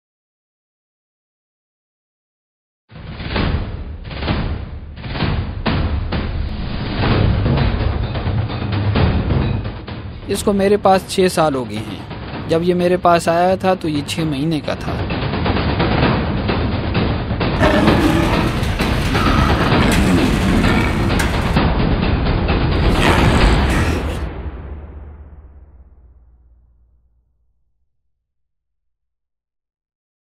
Capture Snow Leopard Promo
This is a promo is the continuation of a teaser of the documentary film about the life of a capture snow leopard in the Pakistan Himalaya  basin city Abbottabad, Khyber Pakhtoon Khawa province.

A real film about the life of the capture snow leopard will be coming soon.

Pakistan is the home of the endangered beautiful big cat, snow leopard.